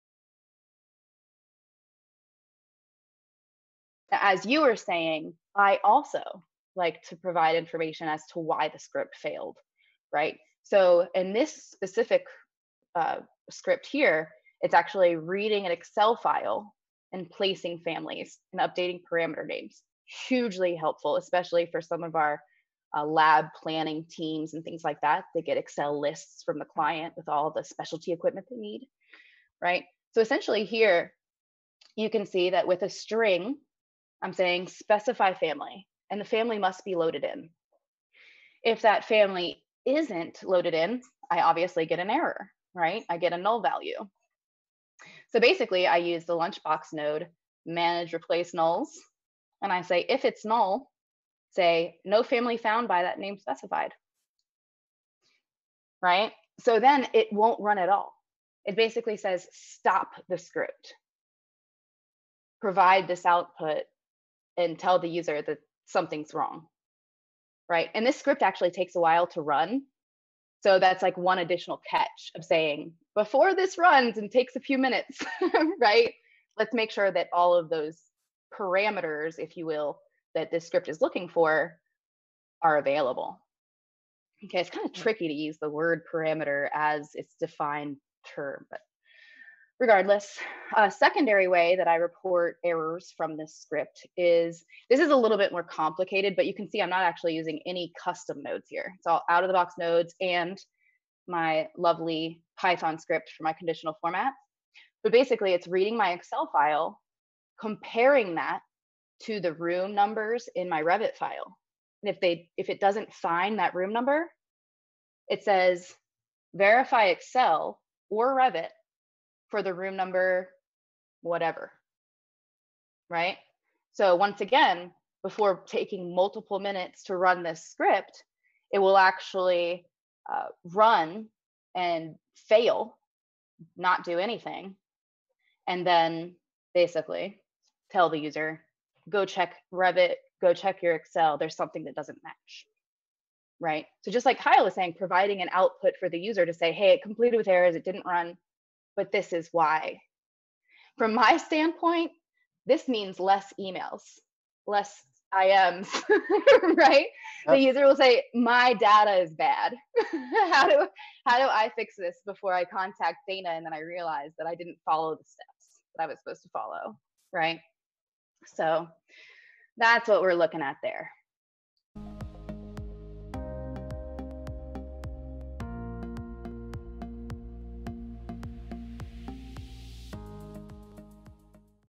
How to Handle Errors with Dynamo Player
A top issue with using Dynamo Players is lacking the ability to report errors.  But not anymore! Watch this highlight from our video if you want to help your users engage with dynamo player.